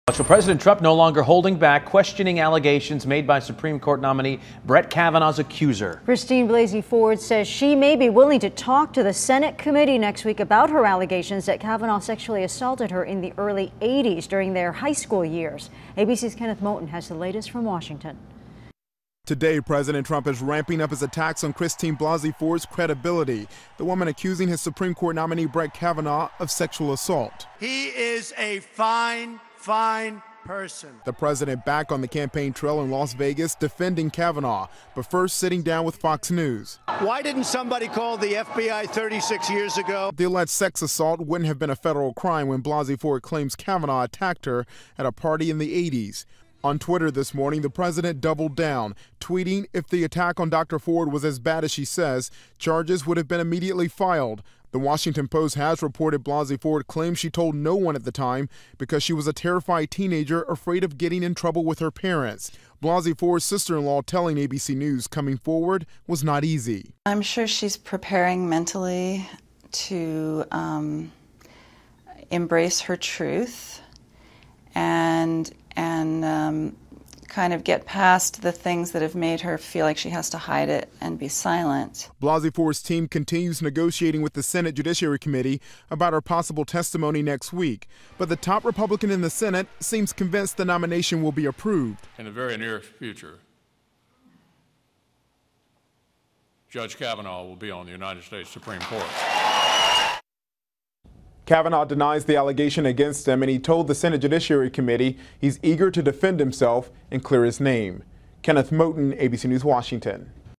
President weighs in on Kavanaugh accuser
Trump spoke out.